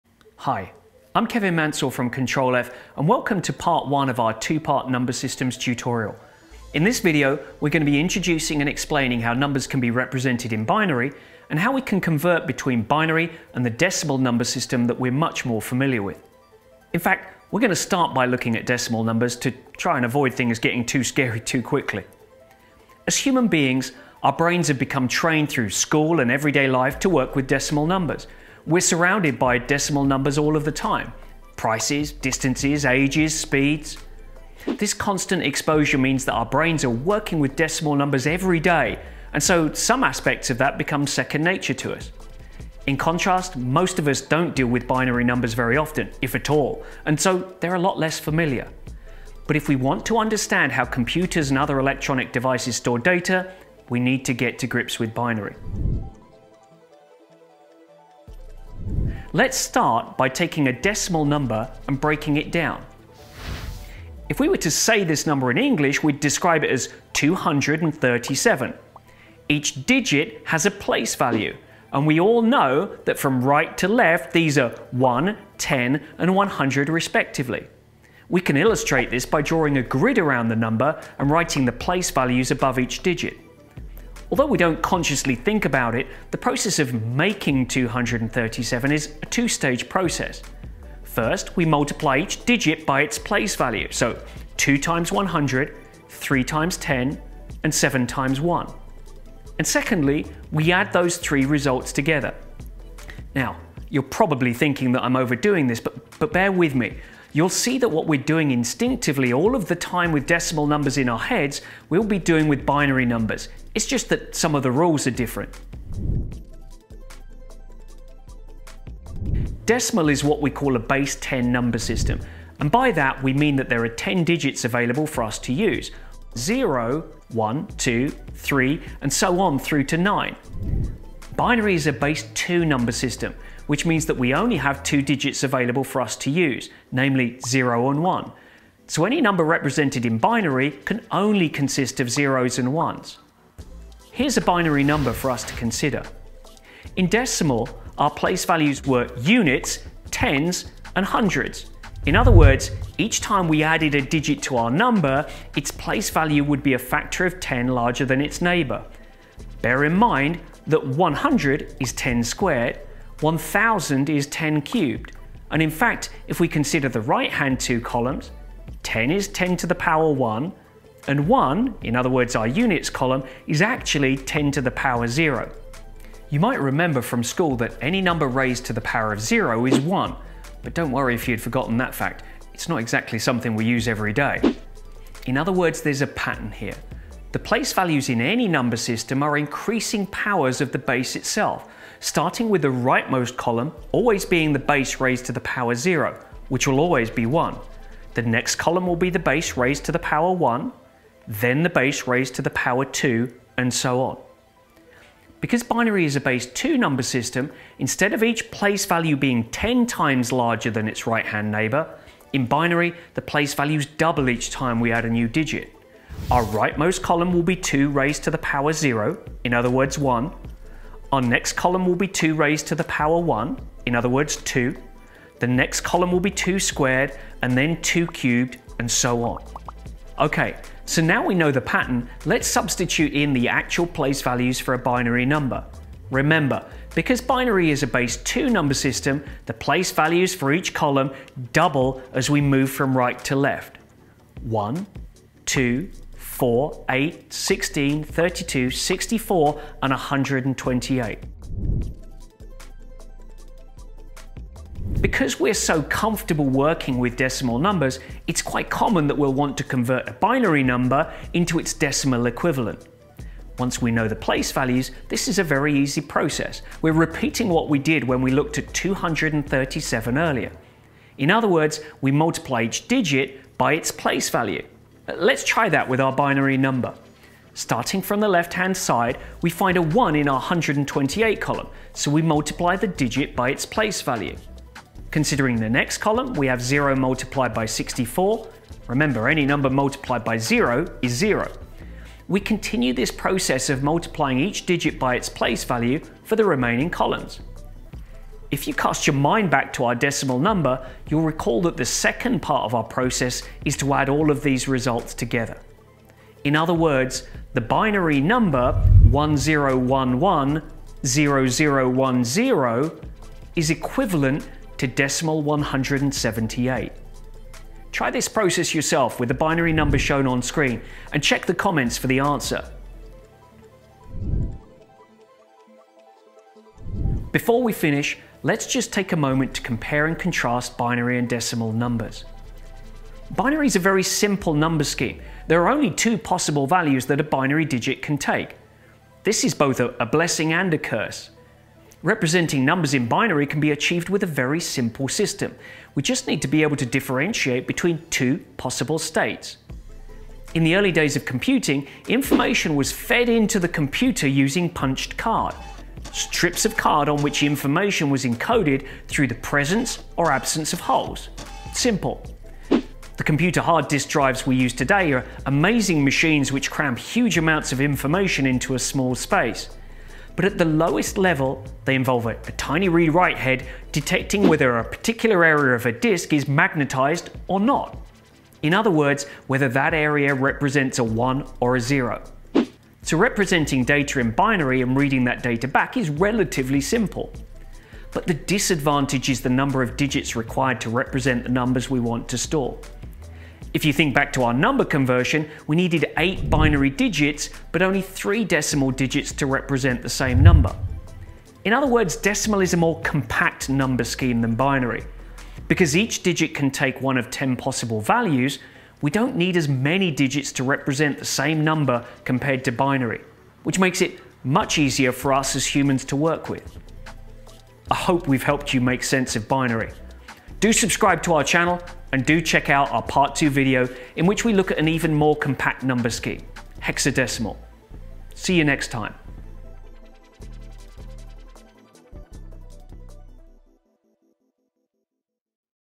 Binary Explained Simply - Number Systems Tutorial (Part 1 of 2)
In the first of our two part number systems tutorial we explain, in a simple step-by-step way, how the binary number system works and how to convert binary numbers into decimal.

Note - strictly speaking, when writing and presenting a binary number we should prefix it with "0b" (or a subscript 2) to make it clear to the reader that the number is in binary and not decimal or some other number system. We didn't mention that in the video, but it's a good habit to get into. In our part 2 of the tutorial you'll see us encouraging the use of a "0x" prefix when presenting hexadecimal numbers for the same reason.